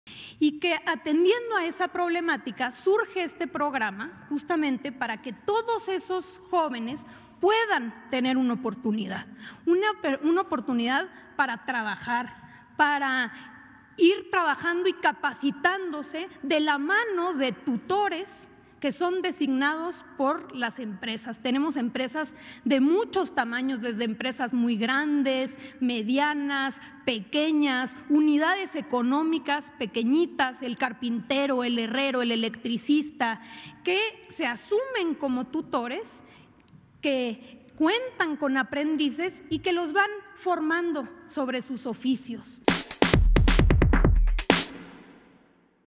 Programa Jóvenes Construyendo el Futuro brinda oportunidades a jóvenes para que puedan capacitarse
Luisa María Alcalde, titular de la Secretaría de Trabajo y Previsión Social, comentó que el Programa Jóvenes Construyendo el Futuro brinda oportunidades a jóvenes que no estudian ni trabajan, para que puedan capacitarse de la mano de tutores que son designados por las empresas, que están afiliadas al programa